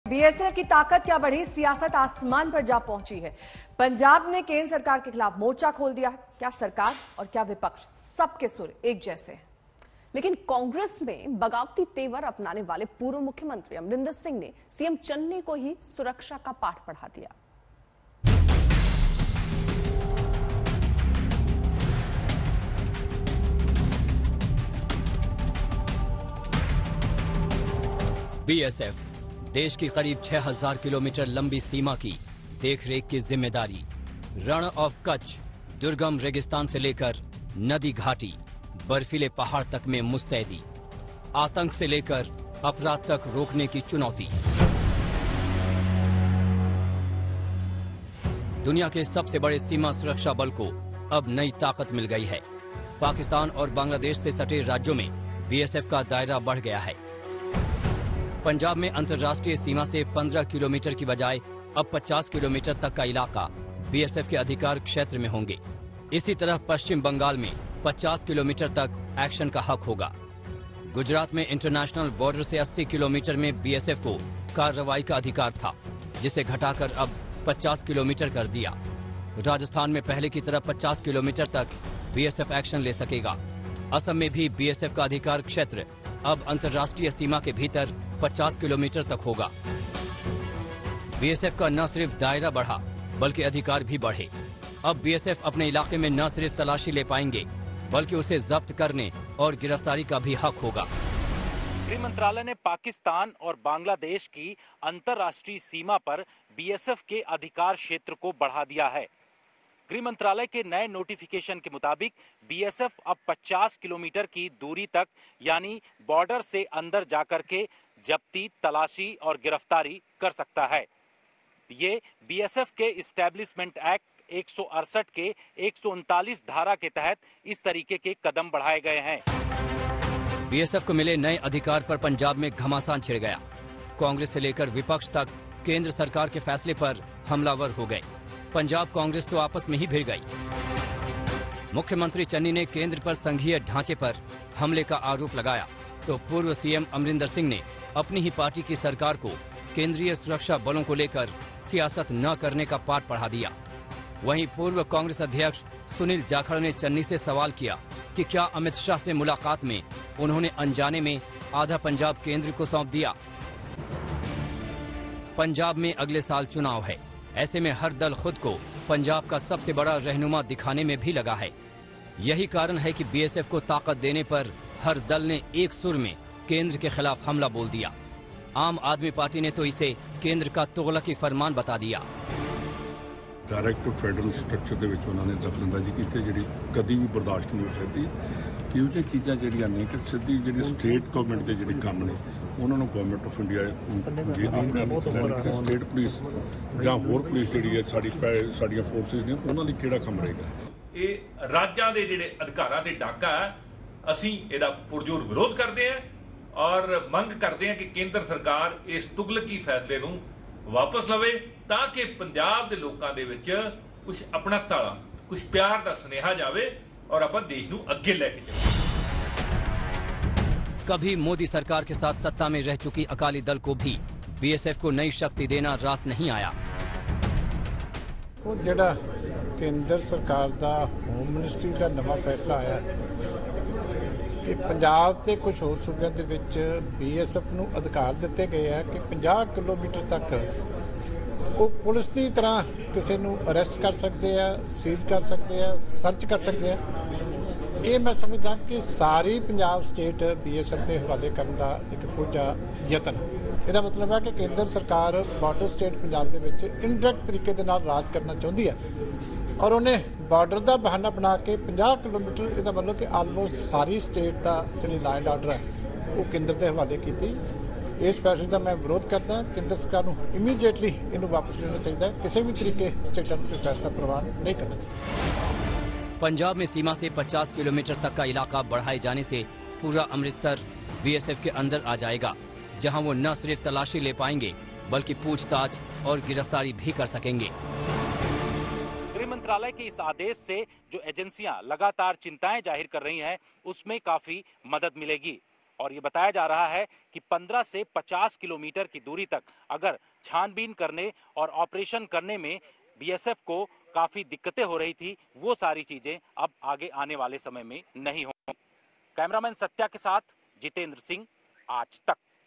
Punjab Congress Politics: BSF की बढ़ती ताकत पर CM Channi और Amrinder Singh में ठनी
Punjab Congress Politics: Punjab Former CM Captain Amrinder Singh Said on Wednesday that the Enhanced power and presence of the Border Security Force in Punjab will only make the State Stronger and Central Armed forces should not be dragged into politics. On the Other hand, the New CM of Punjab Charanjit Singh Chani criticized the center's move to expand BSF's jurisdiction in border states by 50 Kilometres.